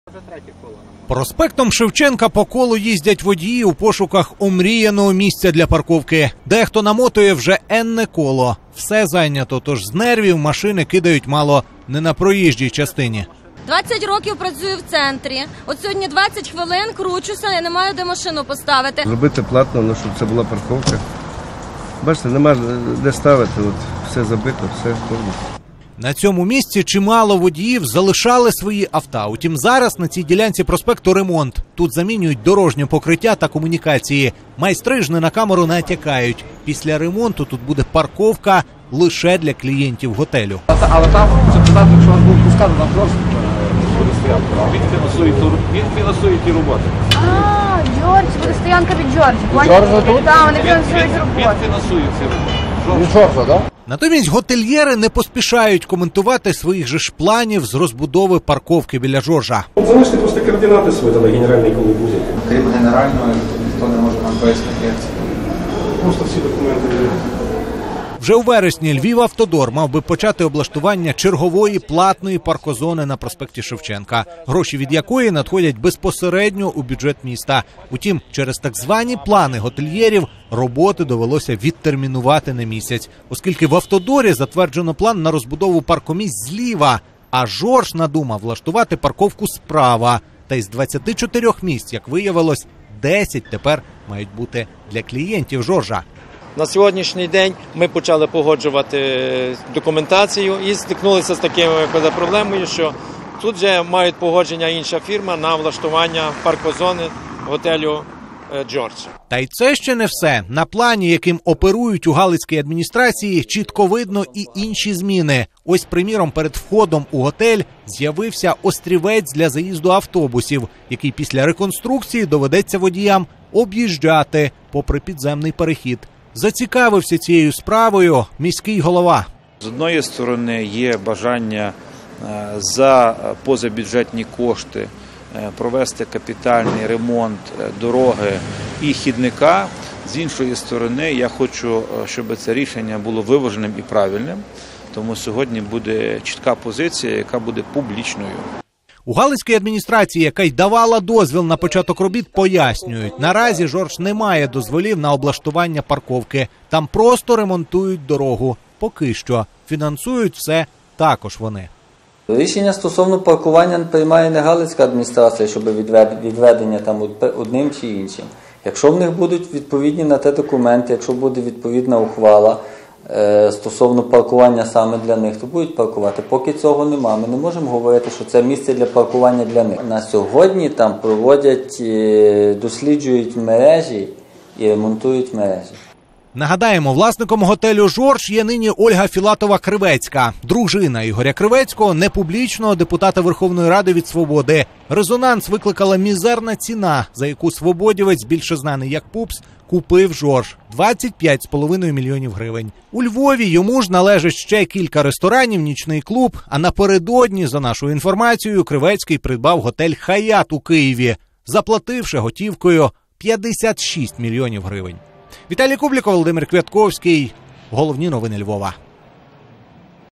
Власник готелю «Жорж» вирішив прихватизувати проїж...
Дружина депутата Верховної Ради від всеукраїнського об’єднання «Свобода» Ігоря Кривецького, більше знаного як Пупс, вирішила прихватизувати проїжджу частину в самісінькому центрі міста. Йдеться про ділянку біля готелю на початку проспекту Шевченка. Там вже почали ремонт. Тут замінюють дорожнє покриття та комунікації. Майстри ж не на камеру натякають, після ремонту тут буде парковка лише для клі...